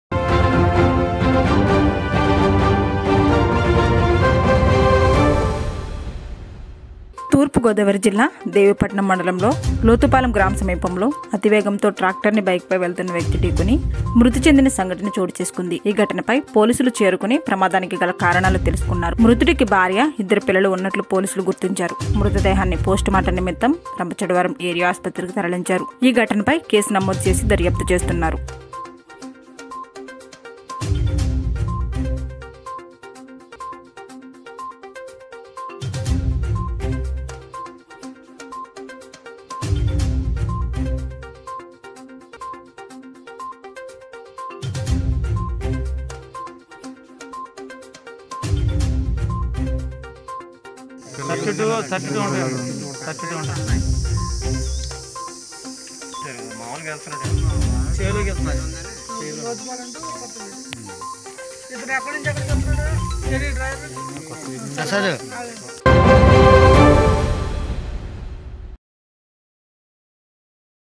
తూర్పుగోదావరి జిల్లా.దేవీపట్నం.లోతుపాలం గ్రామ సమీపంలో అతివేగంతో ట్రాక్టర్ ని డీకొన్న బైక్.
తూర్పుగోదావరి జిల్లా.దేవీపట్నం.లోతుపాలం గ్రామ సమీపంలో అతివేగంతో ట్రాక్టర్ ని డీకొన్న బైక్.సంఘటన స్థలంలో బైక్ పై వెళ్తున్న వ్యక్తి మృతి.